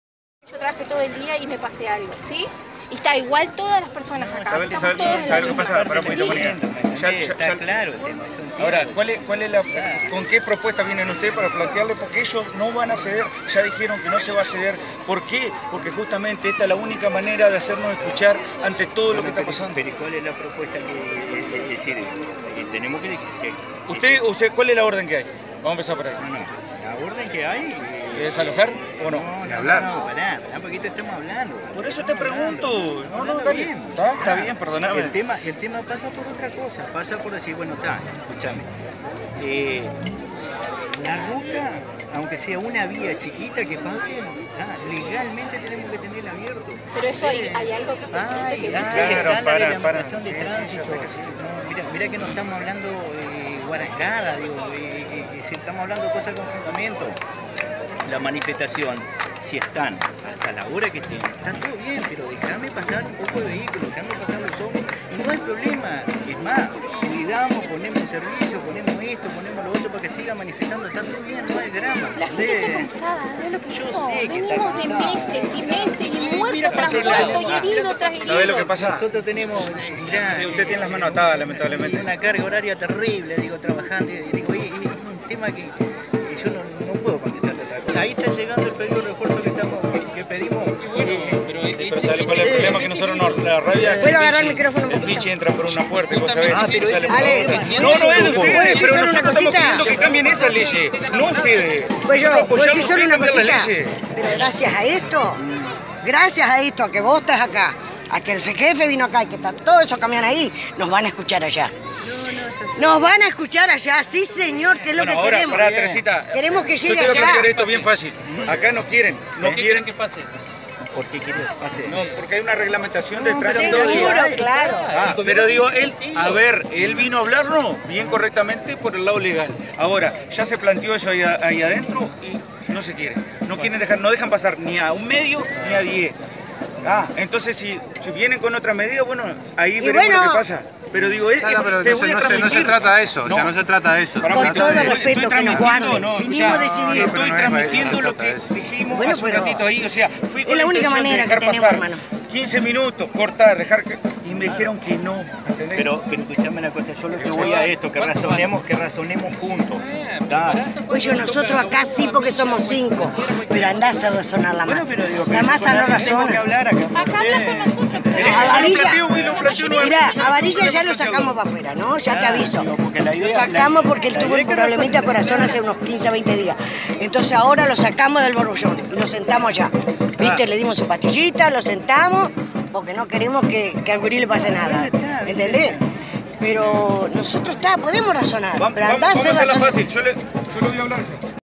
Los manifestantes mantienen postura de no abandonar el corte
Autoridades policiales y manifestantes defienden sus posturas sobre el corte de ruta.